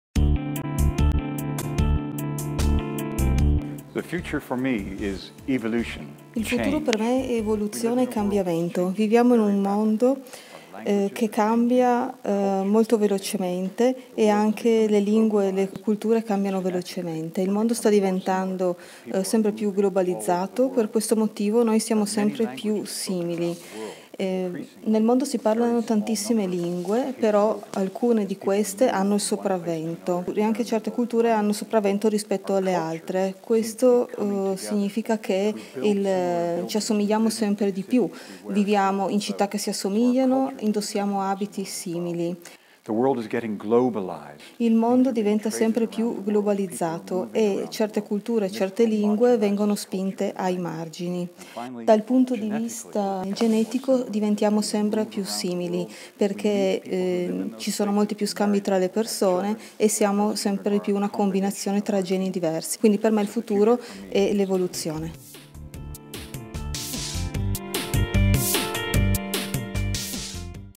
FF2014 - Mark Pagel
TUTTI UGUALI! La fine dell’antropologia: difendere la diversità per combattere l’omologazione che non crea adattamento

MARK PAGEL

Oggi, nel mondo, si parlano circa 6.000 lingue. Dieci di queste sono parlate dal 50% della popolazione mondiale. Secondo l’Unesco la metà delle lingue oggi vive è destinata a scomparire entro la fine del secolo. Bastano questi numeri per cogliere quanto sia avanzato il processo di omologazione delle culture nel mondo e anche quanto alto sia il rischio della prevaricazione. In un mondo piatto, la cultura non potrà che essere piatta, cancellando le diversità, le narrazioni. Dalle culture alla cultura il salto evolutivo è gigantesco e pericoloso. Già Darwin ci ha insegnato che senza diversità non c’è selezione e senza selezione non c’è progresso.

MARK PAGEL: Docente al Dipartimento di Biologia evolutiva dell'Università di Reading, in Gran Bretagana, è membro della Royal Society. Svolge attività di docenza presso il prestigioso Santa Fe Institute, negli Stati Uniti, dove è membro del Consiglio della Scienza. Ha pubblicato numerosi saggi in tema di genetica evolutiva, comportamento, linguistica, tra cui: Wired for Culture: The Natural History of Human Cooperation (2012). Suoi interventi sono ospitati dalle più autorevoli testate internazionali: The New York Times, Scientific American, National Public Radio, National Geographich, the Economist, Wired, New Scientist.